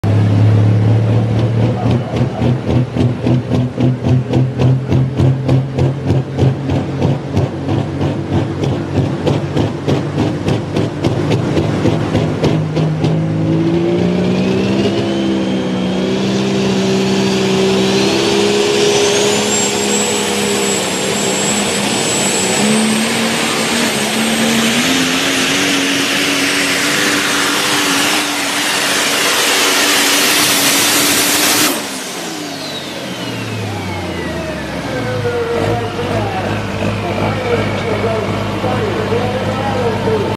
Nick Headley and Binder Edition took the RWYB class at New Centerville 6-30-2023
Nick Headley and Binder Edition took the RWYB class at New Centerville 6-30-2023.  Nick pulled 311.96.  Full results at fullpullproductions.com